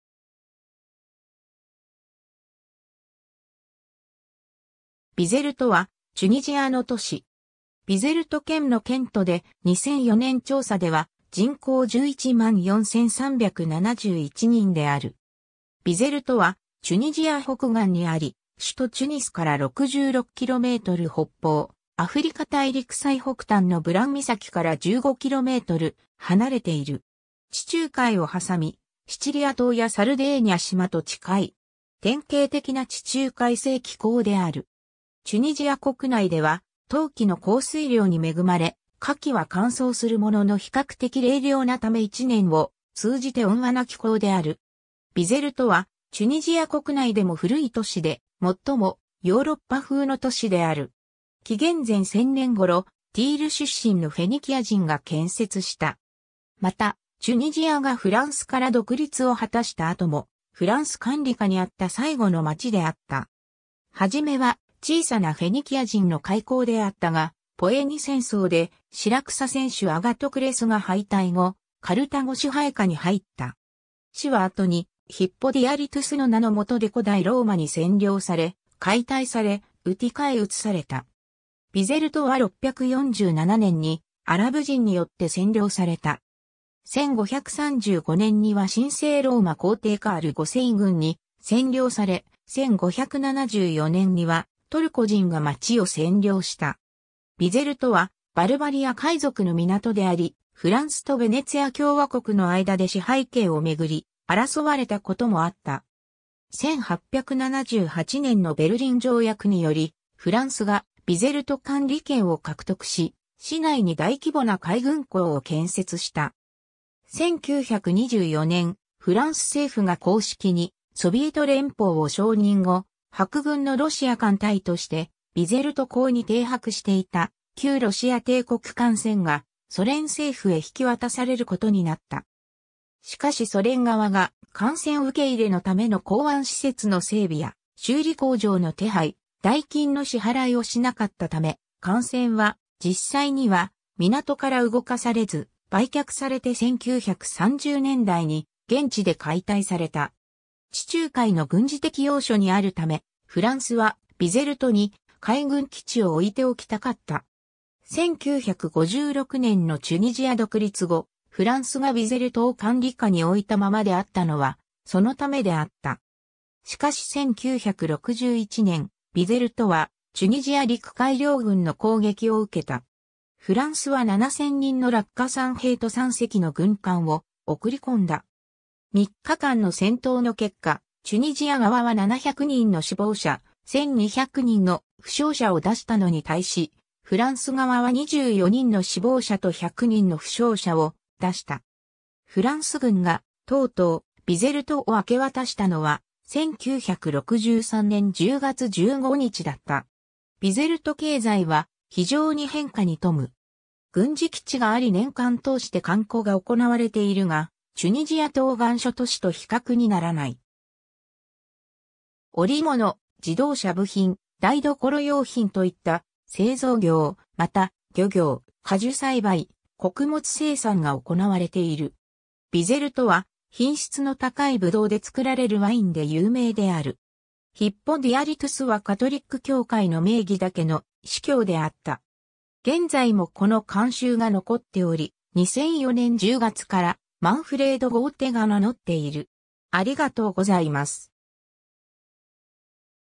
ビゼルト
ビゼルト （BizerteまたはBizerta、アラビア語: بنزرت; /Banzart、イタリア語:Biserta）は、チュニジアの都市。
ビゼルト県の県都で、2004年調査では人口114,371人である。
 ビゼルトはチュニジア北岸にあり、首都チュニスから66km北方、アフリカ大陸最北端のブラン岬から15km離れている。
地中海を挟み、シチリア島やサルデーニャ島と近い。
 典型的な地中海性気候である。
チュニジア国内では冬季の降水量に恵まれ、夏季は乾燥するものの比較的冷涼なため一年を通じて温和な気候である。
 ビゼルトはチュニジア国内でも古い都市で、最もヨーロッパ風の都市である。
紀元前1000年頃、ティール出身のフェニキア人が建設した。
また、チュニジアがフランスから独立を果たした後もフランス管理下にあった最後の町であった。
 初めは小さなフェニキア人の海港であったが、ポエニ戦争でシラクサ僭主アガトクレス（en:Agathocles）が敗退後、カルタゴ支配下に入った。
市は後にヒッポ・ディアリトゥス（Hippo Diarrhytus）の名のもとで古代ローマに占領され、解体され、ウティカへ移された。
 ビゼルトは647年にアラブ人によって占領された（このときにビゼルトの名が与えられた）。
1535年には神聖ローマ皇帝カール5世軍に占領され、1574年にはトルコ人が町を占領した。
ビゼルトはバルバリア海賊の港であり、フランスとヴェネツィア共和国の間で支配権を巡り争われたこともあった。
 1878年のベルリン条約により、フランスがビゼルト管理権を獲得し、市内に大規模な海軍港を建設した。
 1924年、フランス政府が公式にソビエト連邦を承認後、白軍のロシア艦隊としてビゼルト港に停泊していた旧ロシア帝国艦船がソ連政府へ引き渡されることになった。
しかしソ連側が艦船受け入れのための港湾施設の整備や修理工場の手配、代金の支払いをしなかったため、艦船は実際には港から動かされず、売却されて1930年代に現地で解体された。
 地中海の軍事的要所にあるため、フランスはビゼルトに海軍基地をおいておきたかった。
1956年のチュニジア独立後、フランスがビゼルトを管理下においたままであったのはそのためであった。
しかし1961年、ビゼルトはチュニジア陸海両軍の攻撃を受けた。
フランスは7,000人の落下傘兵と3隻の軍艦を送り込んだ。
3日間の戦闘の結果、チュニジア側は700人の死亡者、1,200人の負傷者を出したのに対し、フランス側は24人の死亡者と100人の負傷者を出した。
フランス軍がとうとうビゼルトを明け渡したのは、1963年10月15日だった。
 ビゼルト経済は非常に変化に富む。
軍事基地があり年間通して観光が行われているが、チュニジア東岸諸都市と比較にならない。
織物、自動車部品、台所用品といった製造業、また漁業、果樹栽培、穀物生産が行われている。
ビゼルトは、品質の高いブドウで作られるワインで有名である。
 ヒッポ・ディアリトゥスはカトリック教会の名義だけの司教であった（en:Titular See）。
現在もこの慣習が残っており、2004年10月からマンフレード・ゴーテが名乗っている。